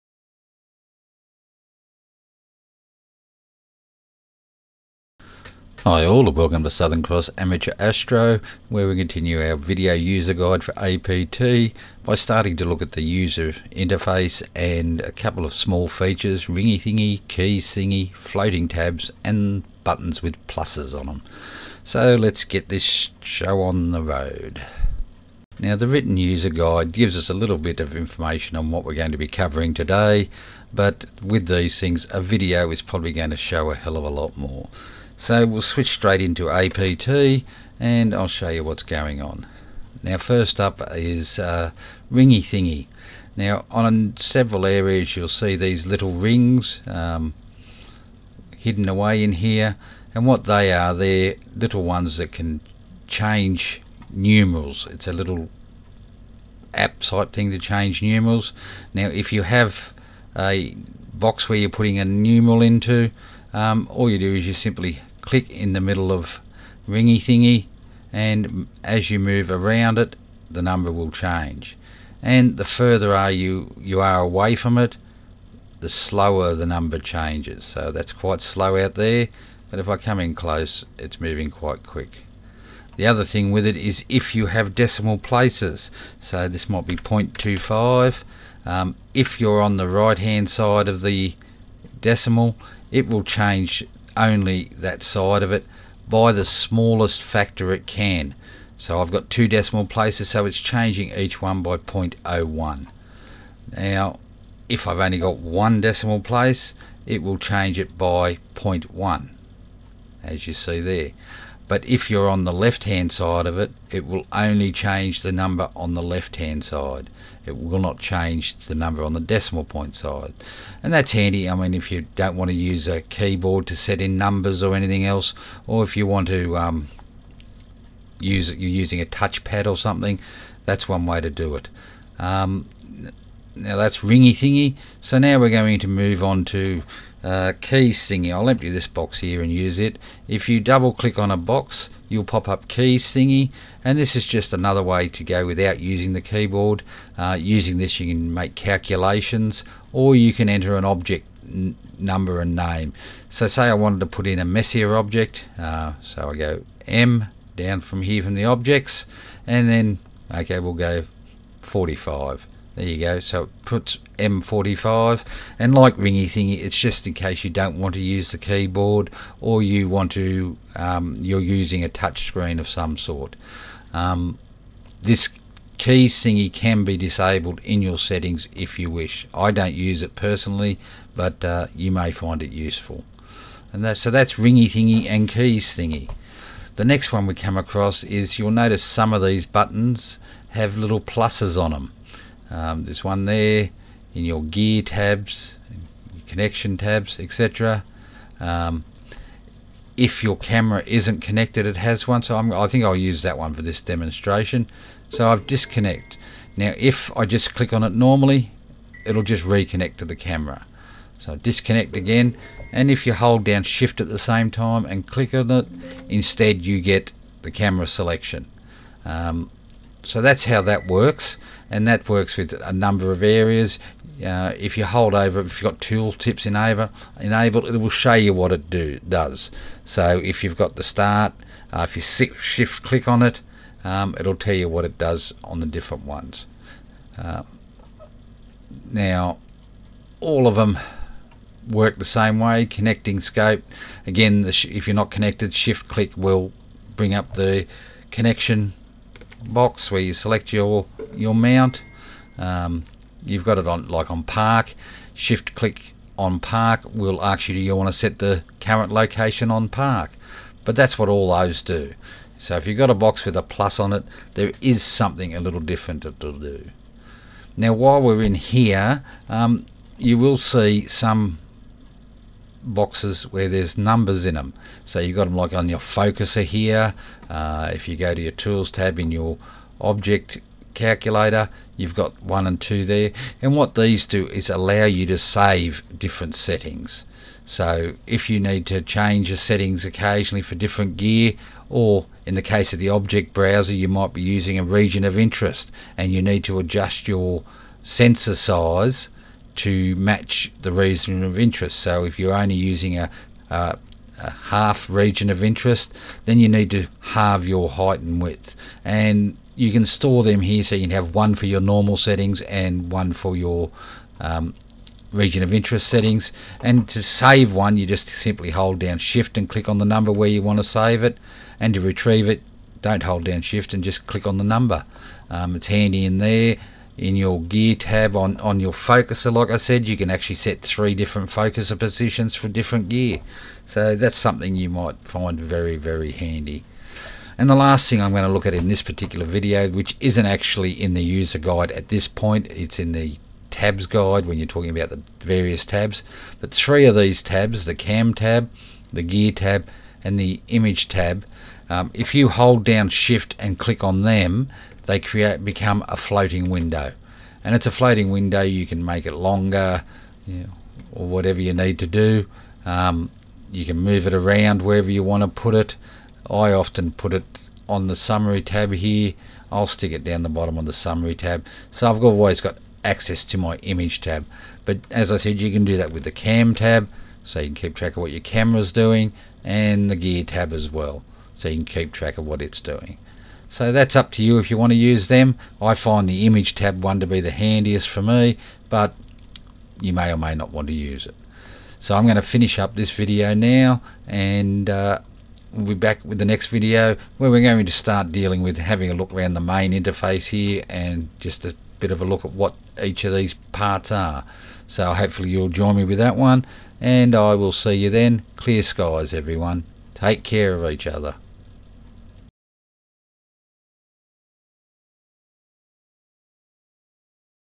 APT Video User Guide - Interface (some small features)
Astro Photography Tool Video User Guide
Application Interface
Ringy Thingy, Keys Thingy, Floating Tabs, '+' Buttons & Quick Profiles/Settings/Bookmarks

Support APT!
Having alternatives drives progress!

00:00 Intro
00:37 Ringy Thingy
02:10 Keys Thingy
03:05 '+' Buttons
04:28 Quick Profiles/Settings/Bookmarks
05:36 Floating Tabs